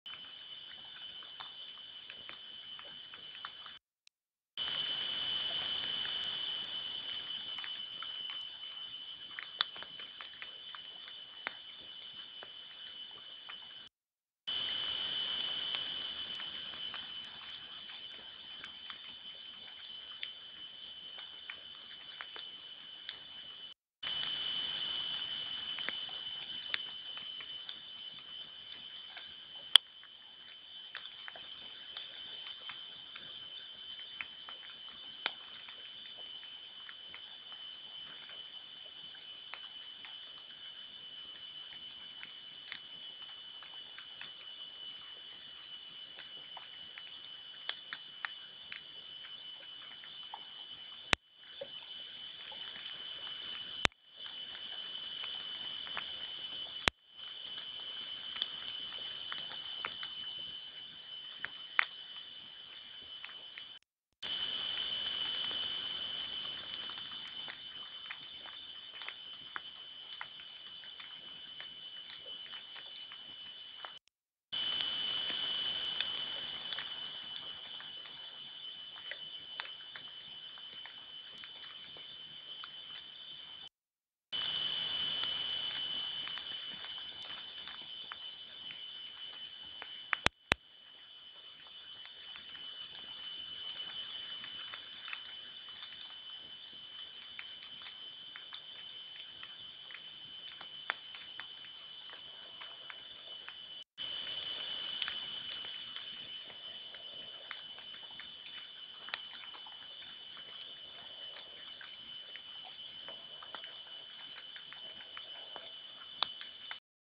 Karu-Bear Eating at Center-30 May 2013
A bear stands at center & enjoys a meal.